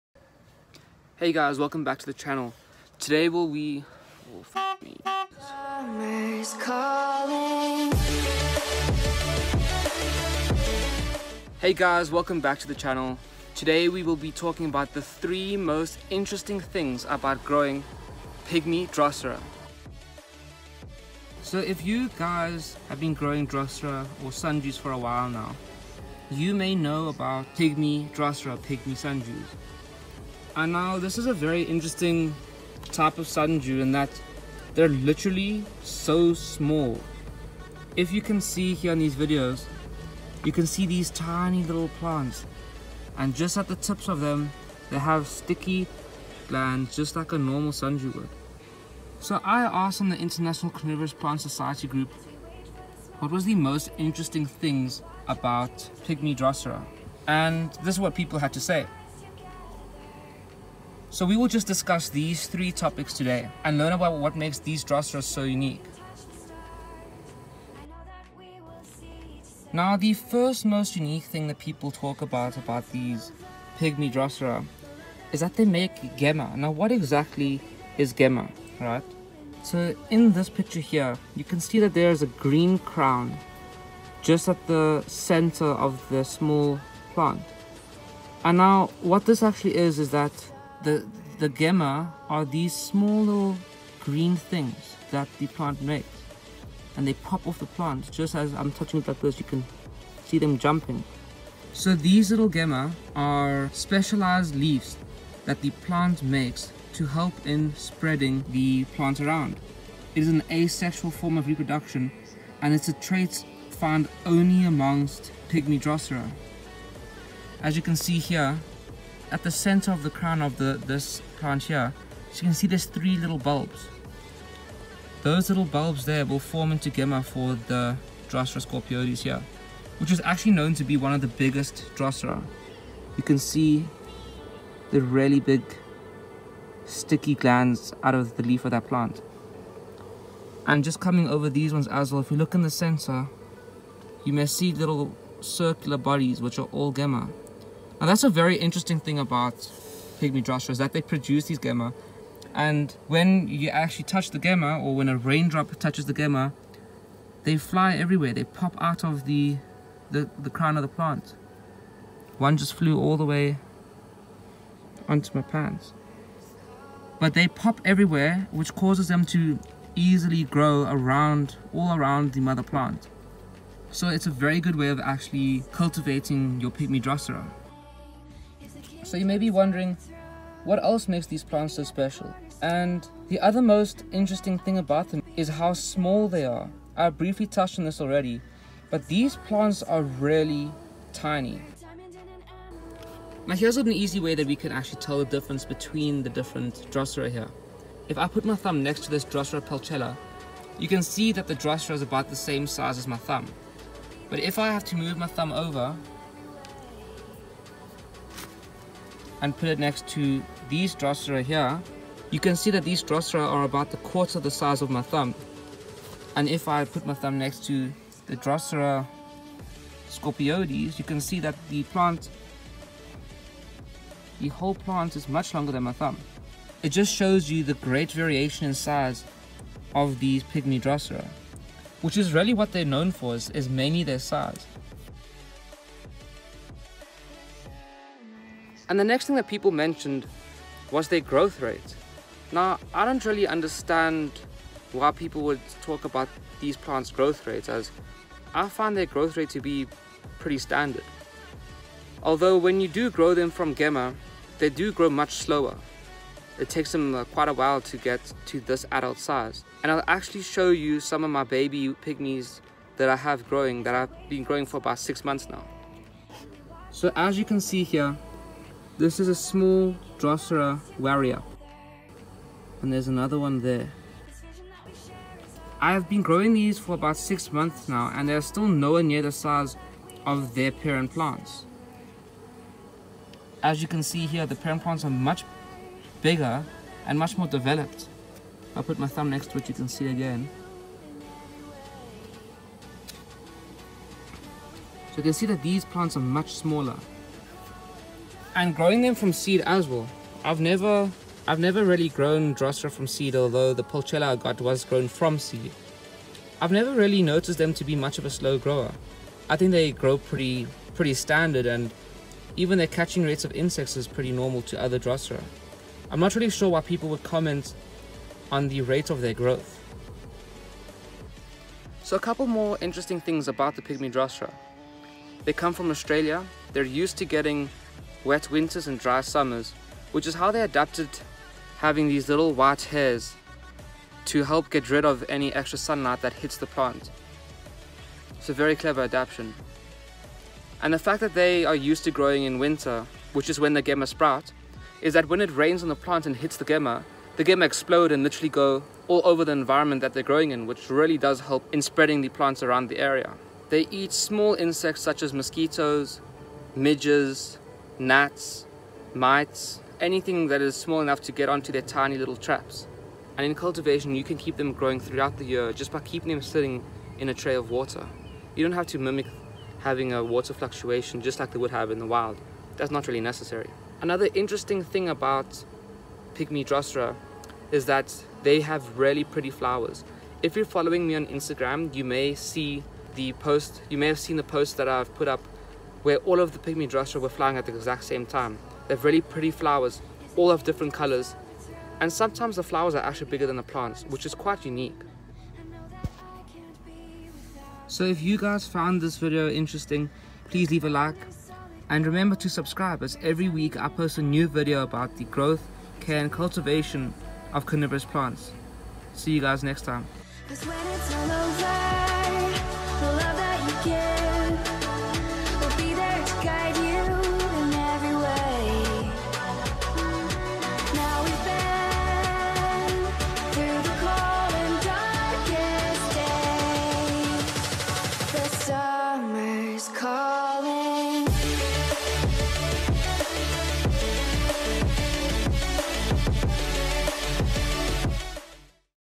Pygmy Sundew - What Exactly Makes These Plants Interesting?
What do people think is the most interesting thing about pygmy Drosera? Their size, their reproductive strategies (gemmae) or their growth speeds? Today I discuss my opinions on the subject with you!

Question of the Day:
What does pygmy even mean?

Some of the links mentioned in the above video and/or description may generate sales commission towards this channel.